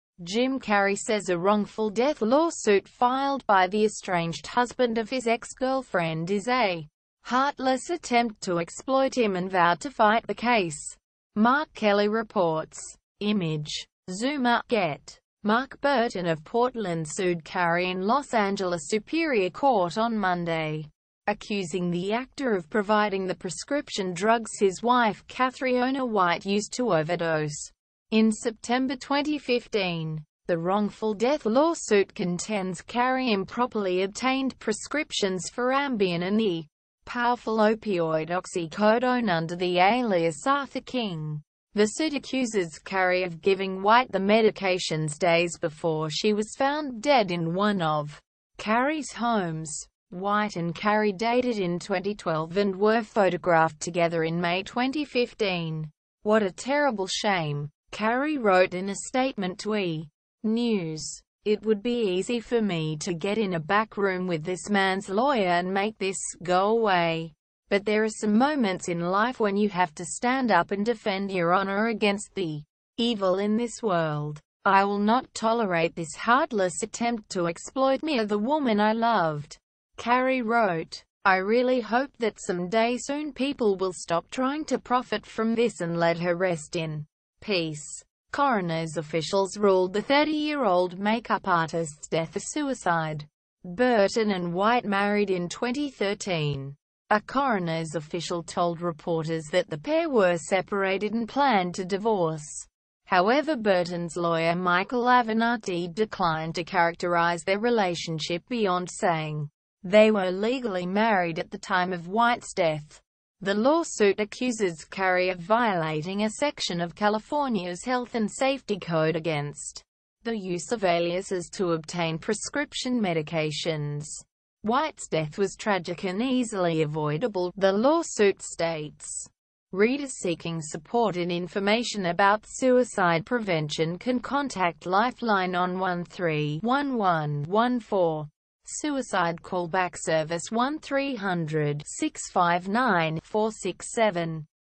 Jim Carrey slams ‘evil’ suicide lawsuit
Jim Carrey says a wrongful death lawsuit filed by the estranged husband of his ex-girlfriend is a heartless attempt to exploit him and vowed to fight the case. Mark Kelly reports. Image: Zuma/GettyMark Burton of Portland sued Carrey in Los Angeles Superior Court on Monday, accusing the actor of providing the prescription drugs his wife Cathriona White used to overdose in September 2015.The wrongful death lawsuit contends Carrey improperly obtained prescriptions for Ambien and the powerful opioid oxycodone under the alias Arthur King.The suit accuses Carrey of giving White the medications days before she was found dead in one of Carrey’s homes. White and Carrey dated in 2012 and were photographed together in May 2015.“What a terrible shame,” Carrey wrote in a statement to E! News. “It would be easy for me to get in a back room with this man’s lawyer and make this go away, but there are some moments in life when you have to stand up and defend your honour against the evil in this world.